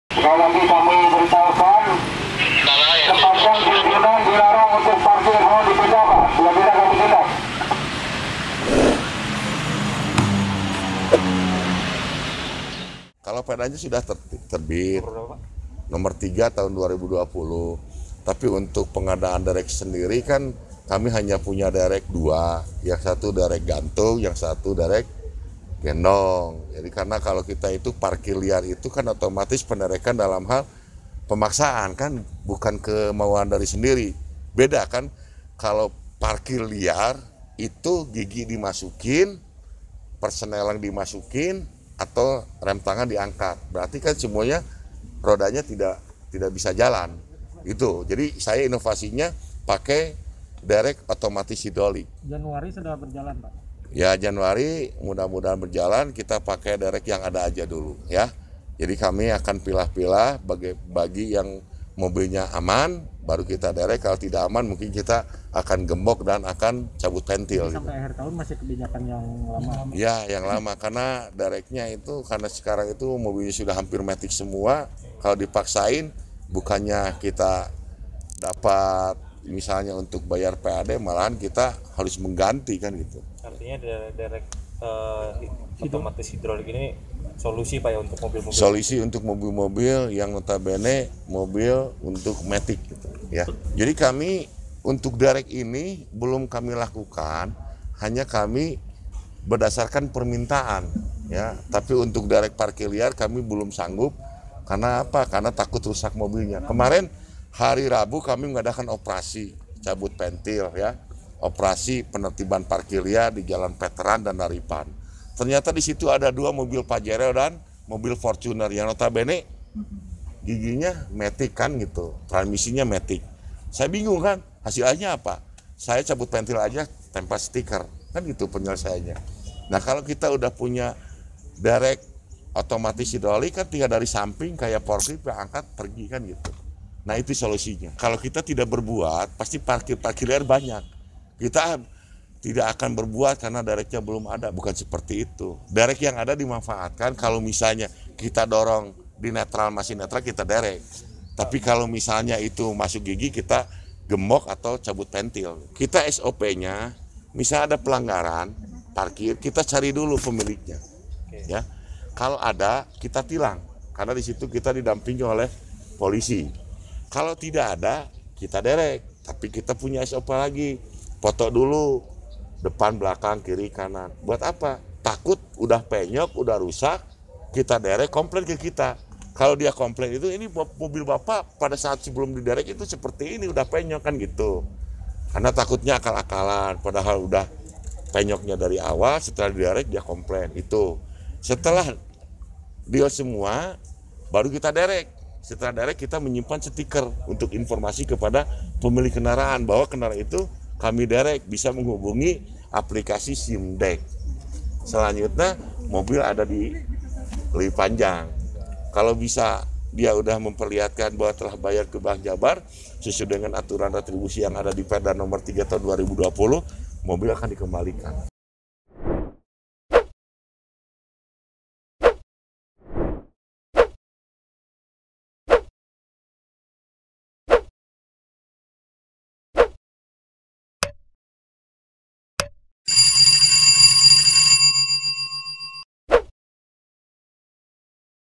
Parkir Liar Masih Marak, Perda Derek Segera Diberlakukan di Kota Bandung
Foto Video : Dok. PDKT Dishub Kota Bandung